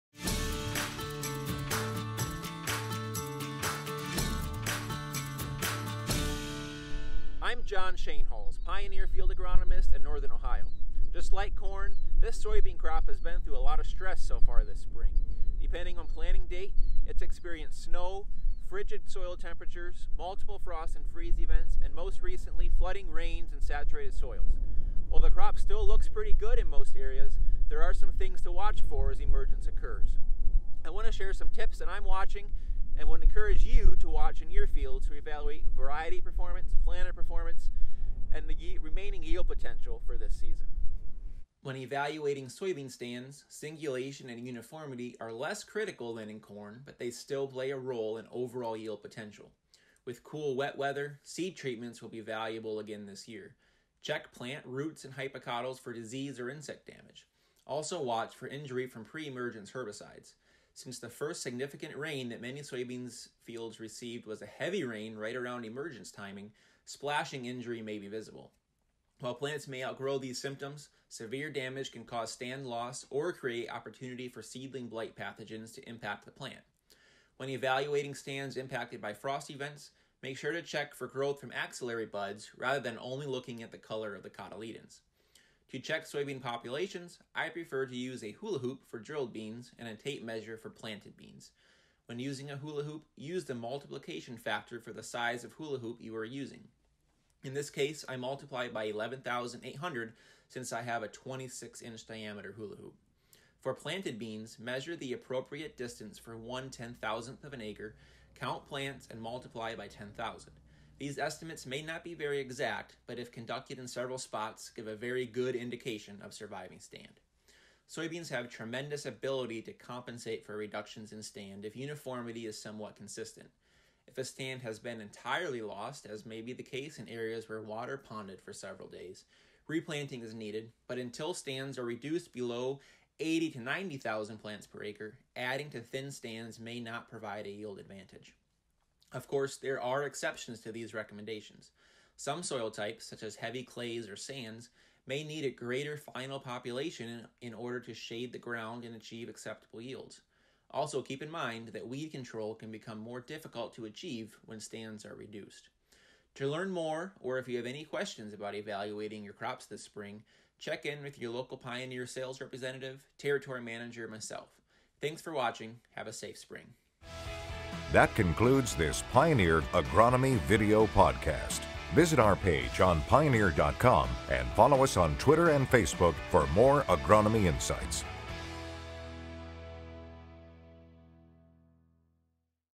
Evaluating Soybean Emergence and Stand
Emergence is a critical time to be out scouting and evaluating your stands to ensure your soybeans are set up for success. In this video, Pioneer Field Agronomist John Schoenhals provides some guidance on things to look out for when you’re scouting your soybean fields evaluating soybean emergence and stands. Singularity and uniform emergence is important, so making sure to apply fungicide where needed, scout for pest and disease pressure, and keeping an eye out for injury from pre-emergence herbicide. When evaluating stands, make sure to check soybean auxiliary buds for frost damage. John also shows you a quick way to count plants to get stand estimates. He also gives replant guidelines, as soybeans are more resilient to stand loss than other crops.